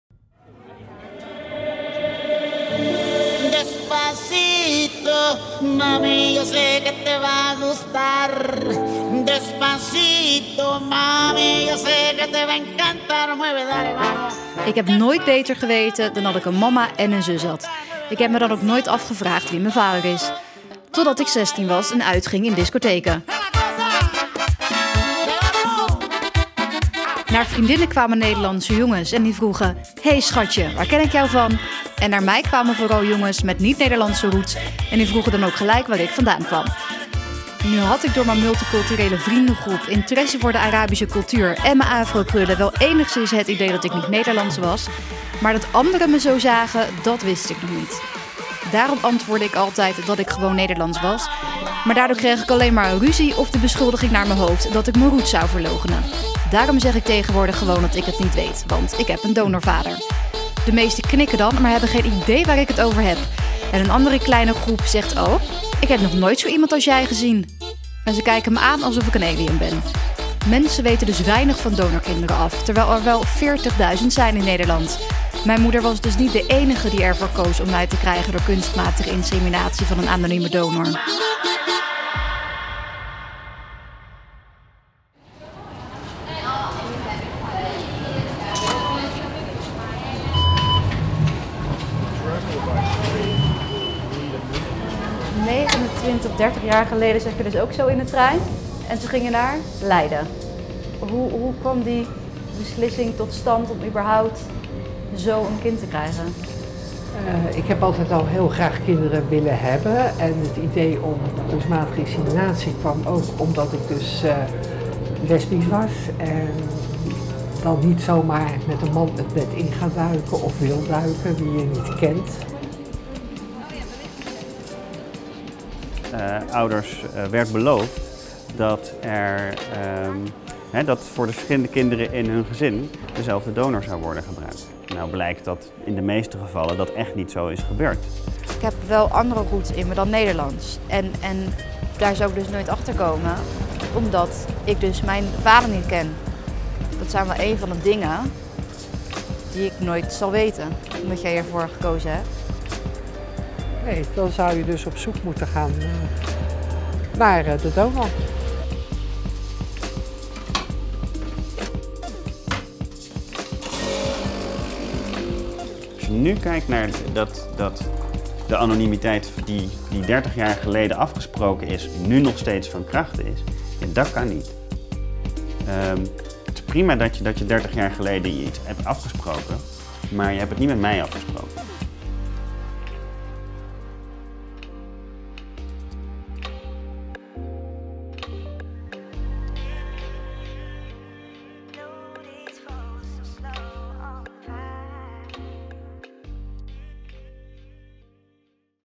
Pilot 'Vader Onbekend'
In Nederland zijn er 40.000 kinderen met een anonieme donorvader. Deze documentaire brengt in beeld wat het betekent om een donorkind te zijn en wat zij kunnen doen om hun biologische vader te vinden.

De documentaire wordt gemaakt door freelance journalisten Willemijn de Koning en Jorinda Teeuwen. Met behulp van een crowdfundingactie op hopen we de financiën rond te krijgen.